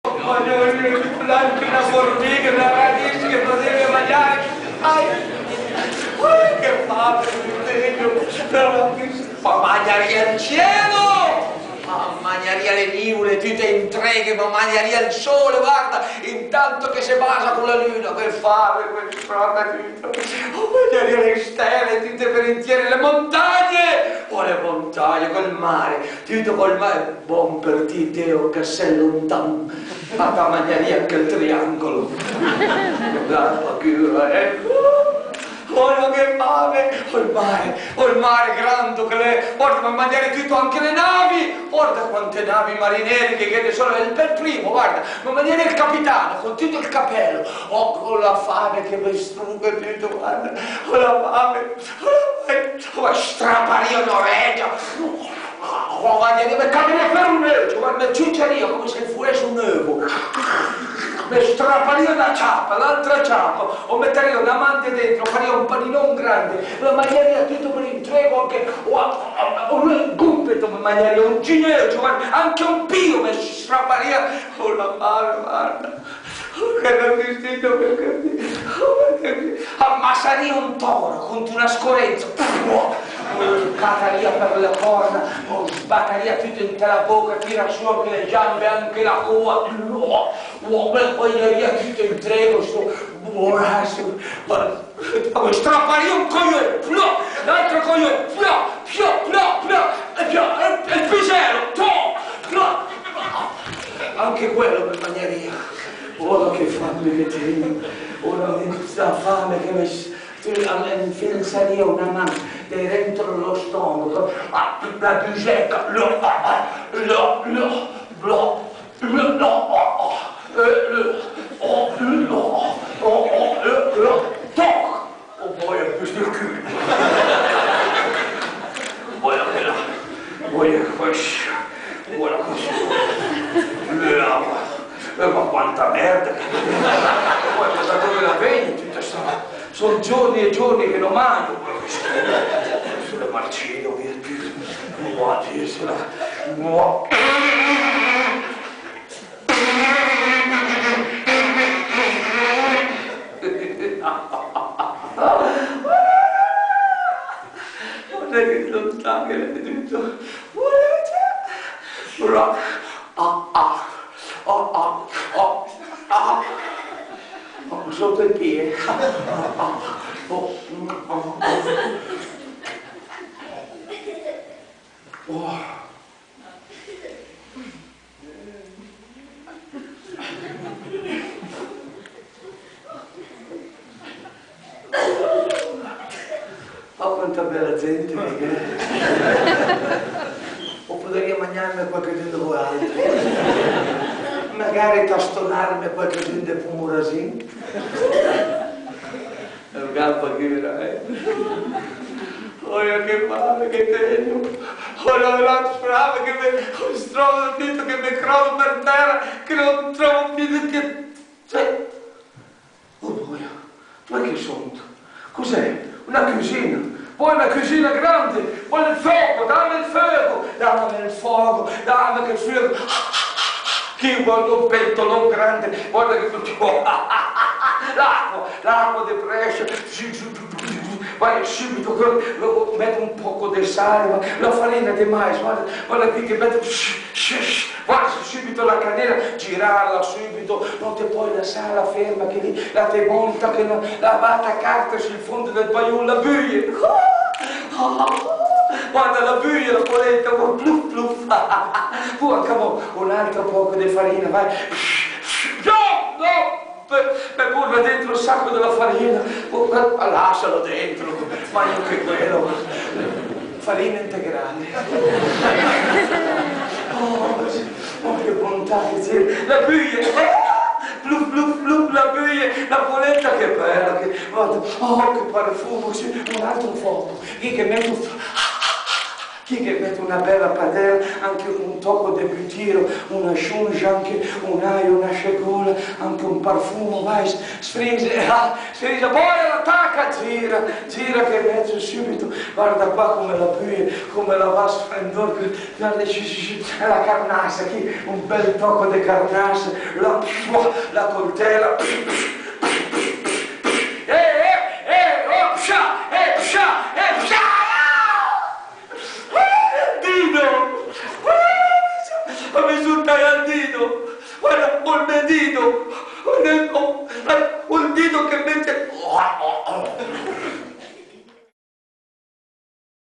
Mario Pirovano al Cinema Rio di Poschiavo #1
L'attore interpreta La fame dello Zanni del Mistero Buffo di Fo.